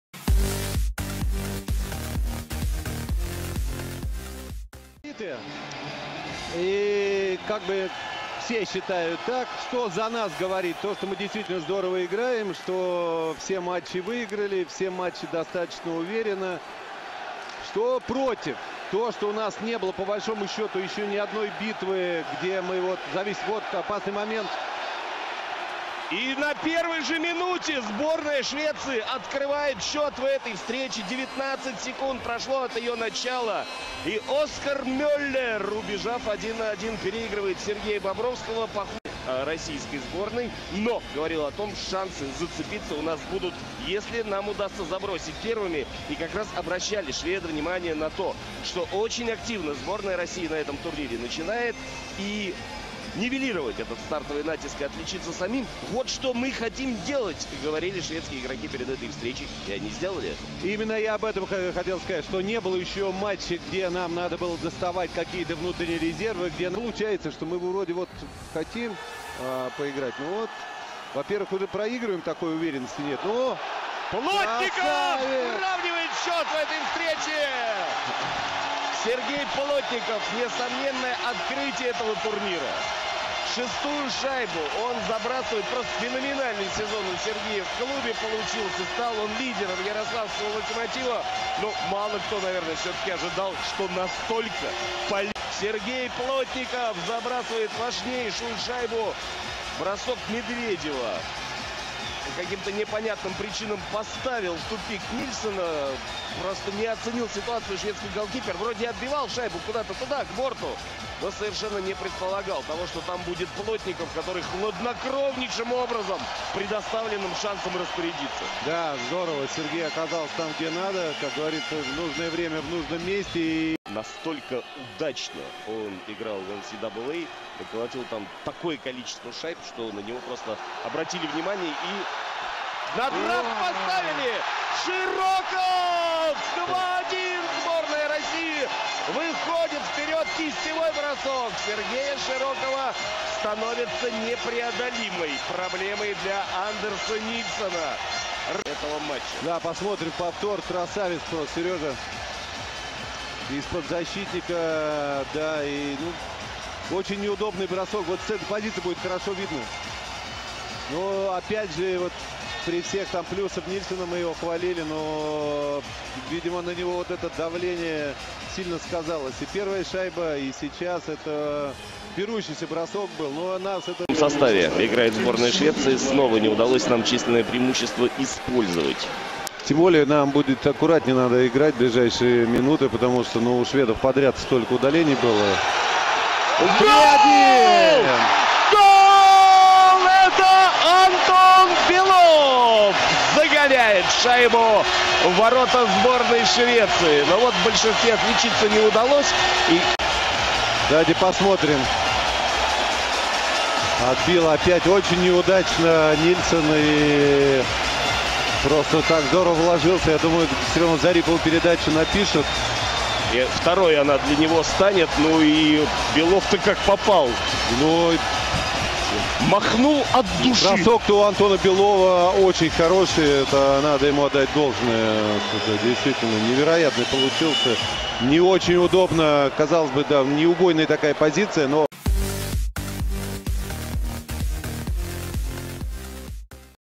Russia vs Sweden 2014-05-24 3-1 WC 2014
2014 IIHF World Championship in Minsk
there you can see all highlights in Russian in good quality.
!!!PLEASE DON*T MIX SPORTS WITH POLITICS OR VICE VERSA THO I GUESS SOMETIMES IT'S IMPOSSIBLE!!!